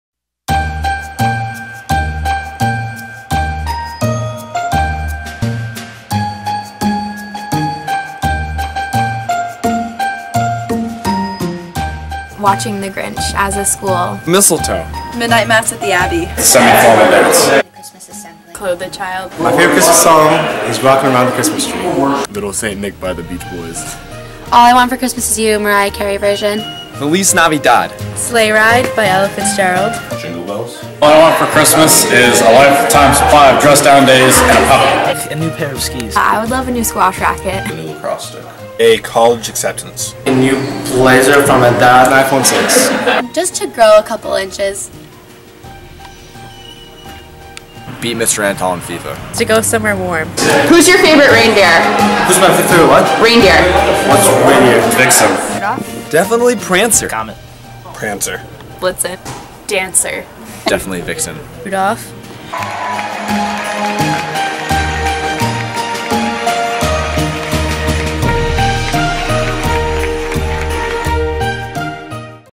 Merry Christmas from Portsmouth Abbey's Red Key Society
Merry Christmas from Portsmouth Abbey's Red Key Society and the Office of Admission! We look forward to reading the applications of our newest Ravens in the new year!